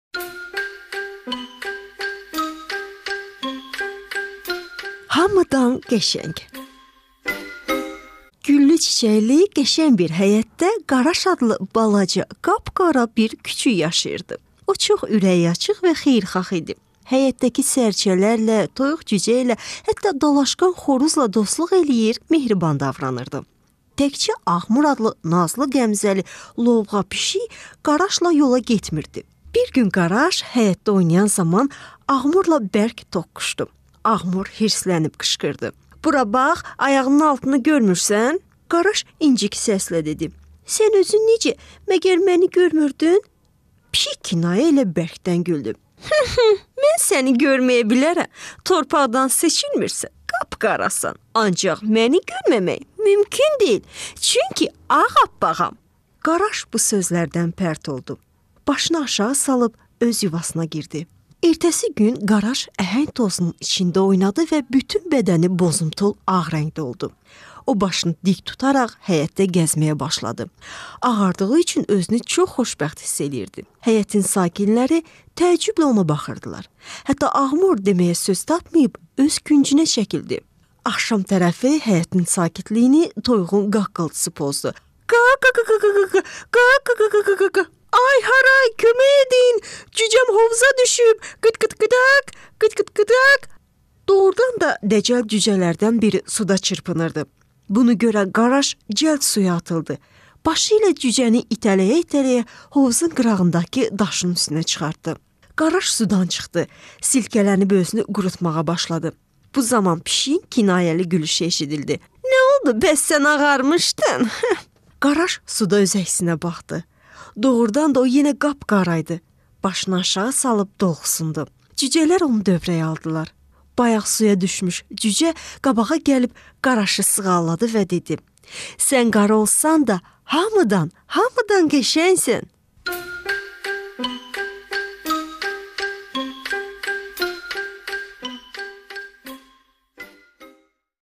Azərbaycan nağılı "Hamıdan qəşəng" (səsli nağıl).
Abunə olun.

Qara it balası və ağ pişiyin nağılı.

Nagillar
Nağıllar
Nagıllar
Nağillar
Nagil
Nağıl
Nagıl
Nağil